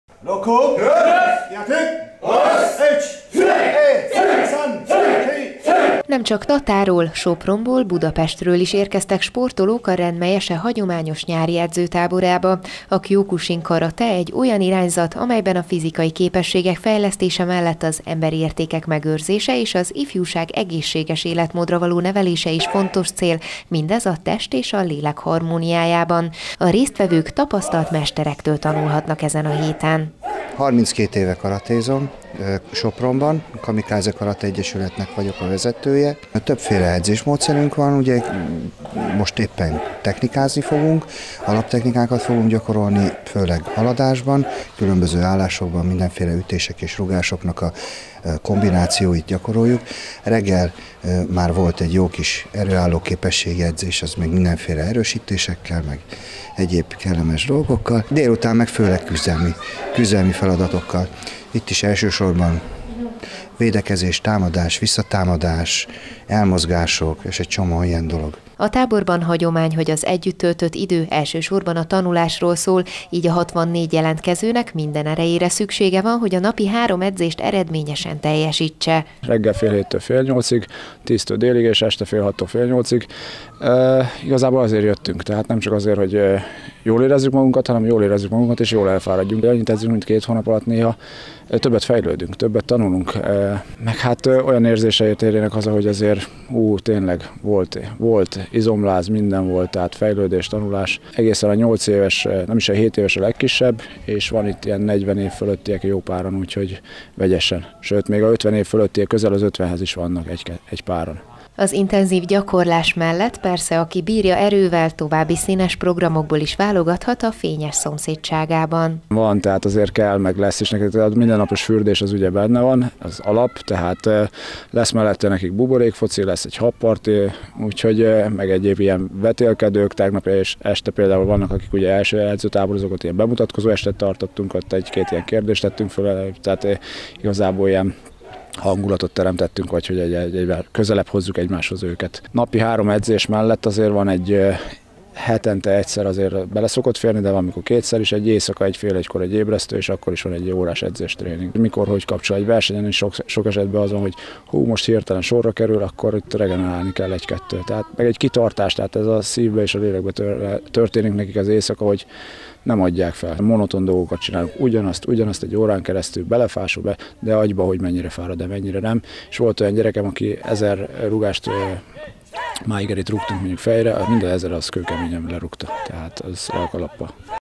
190801 Híradó 03 Karate tábor
A Renmei Kyokushin Karate Sport Egyesület hagyományosan a Fényes Fürdő területén kialakított edzőközpontban és szabadtéren rendezi egyhetes szakmai továbbképzését, táborát gyakorlati oktatással színesítve a programot. Az intenzív, napi 3 edzéssel zajló programba nem csak helyi, de más sportegyesületek is előszeretettel kapcsolódnak.